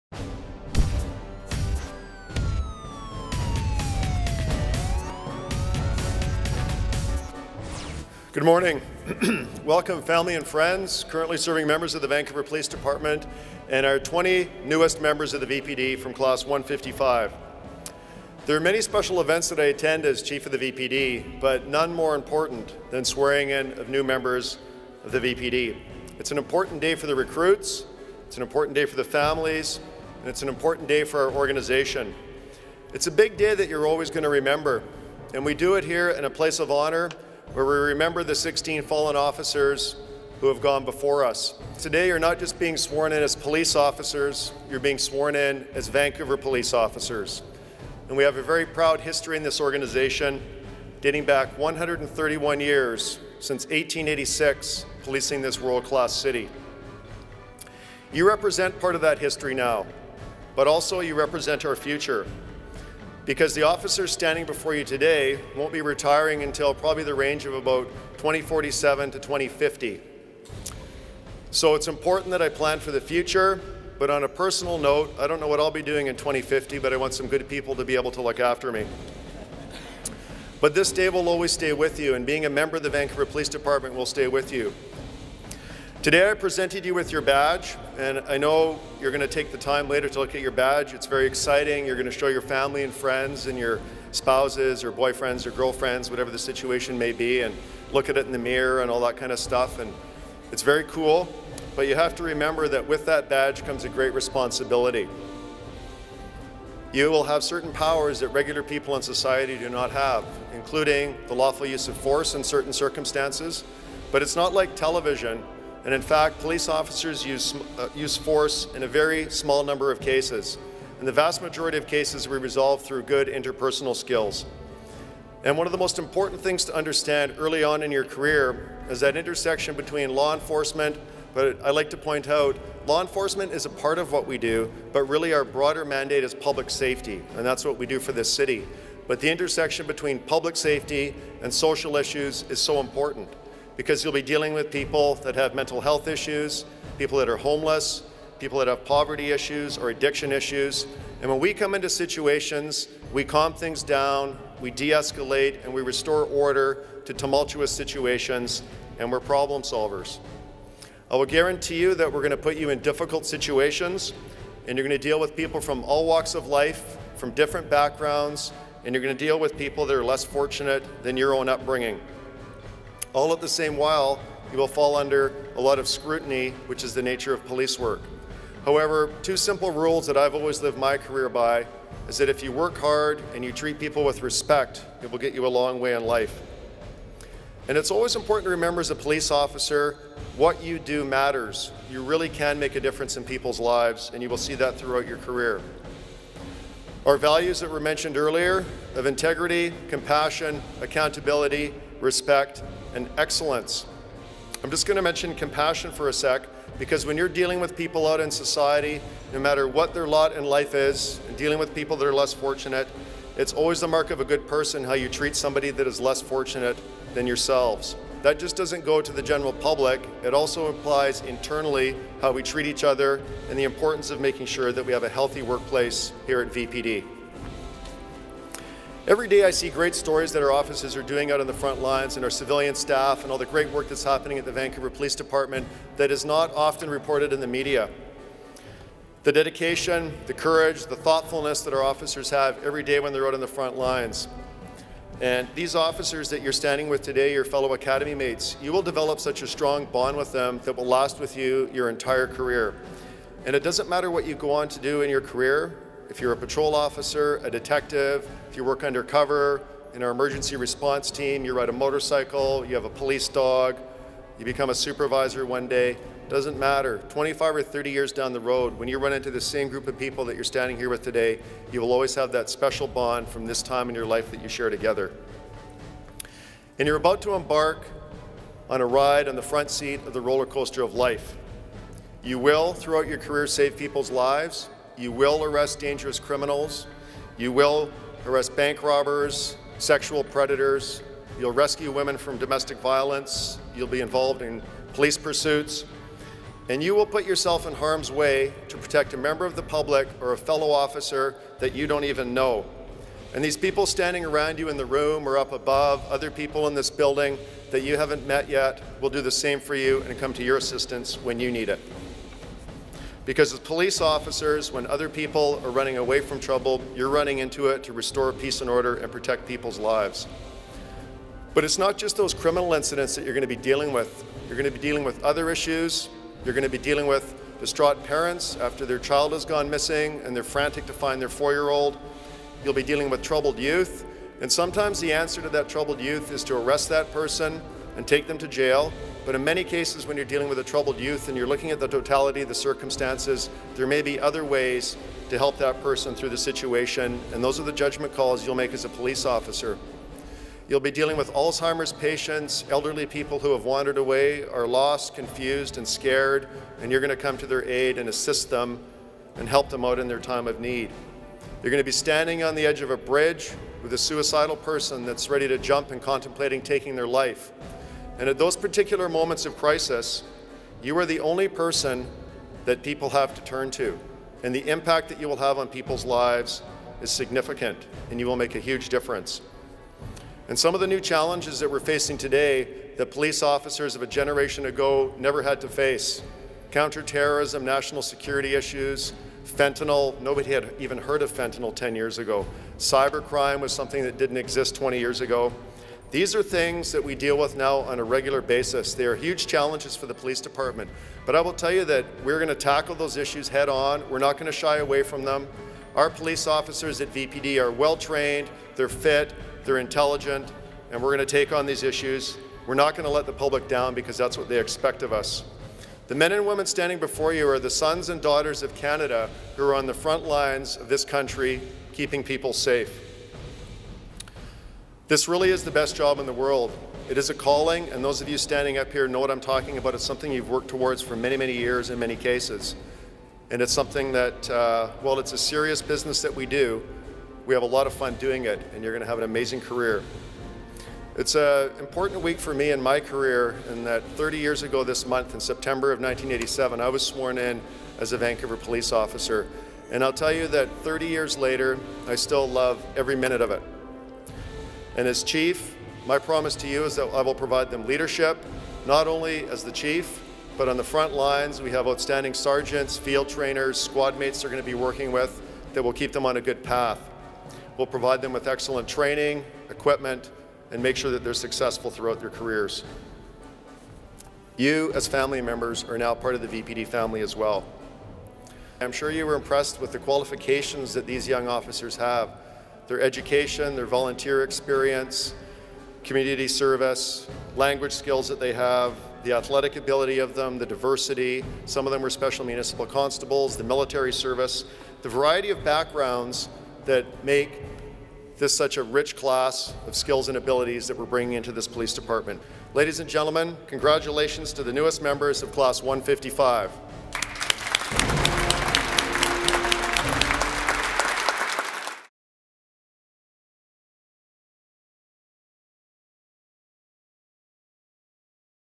Swearing In Ceremony - Recruit Class 155
A recent Swearing In Ceremony welcomed the newest additions to the VPD.